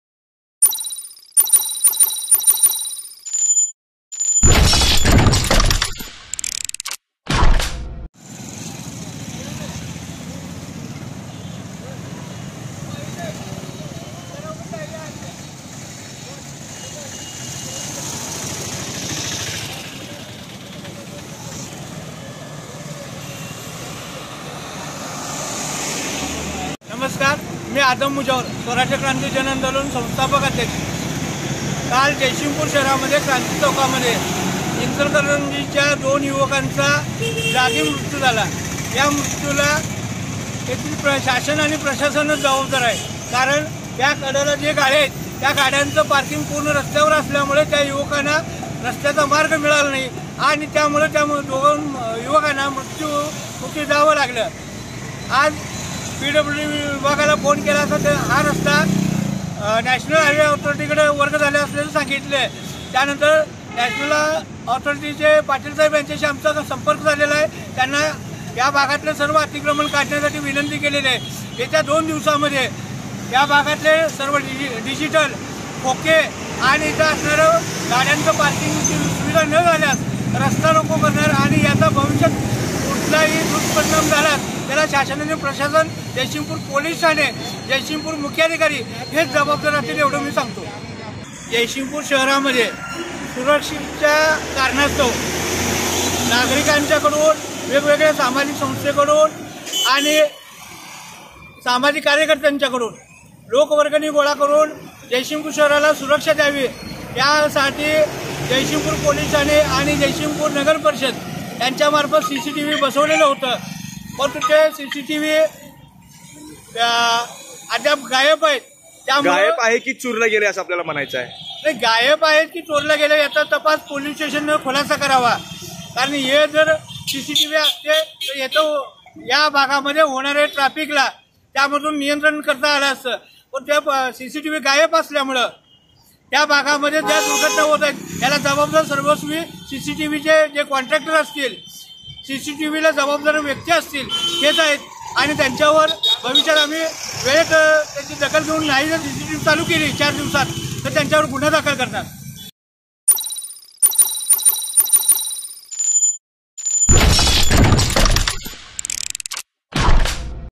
बेशिस्त वाहतूक व डिजिटल बोर्डाच्या विळख्यामुळे अपघात वाढले
*क्रांती चौक डिजिटल मुक्त करा; चौकात ट्राफिक पोलिसांची नेमणूक असावी*